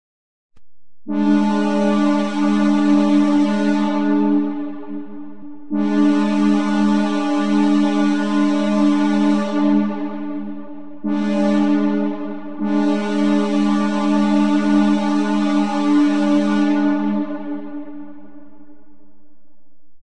Burlington Northern EMD E9 Farewell Horn Synthesizer
This is the horn the E9s used during their farewell trip in 1992.
Horn once again by The SCL Railfan.